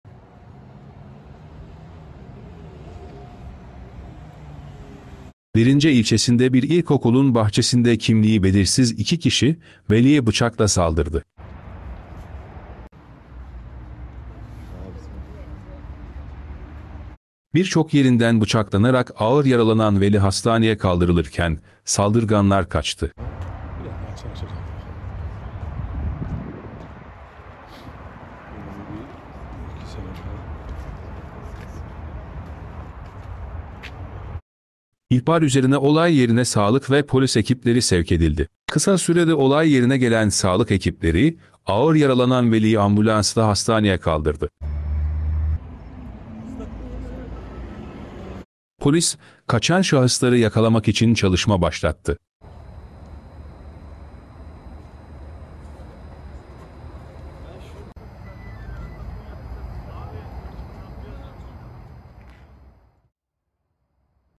OKUL BAHÇESİNDE VELİYE BIÇAKLI SALDIRI
Derince ilçesinde bir ilkokulun bahçesinde kimliği belirsiz 2 kişi, veliye bıçakla saldırdı. Birçok yerinden bıçaklanarak ağır yaralanan veli hastaneye kaldırılırken, saldırganlar kaçtı.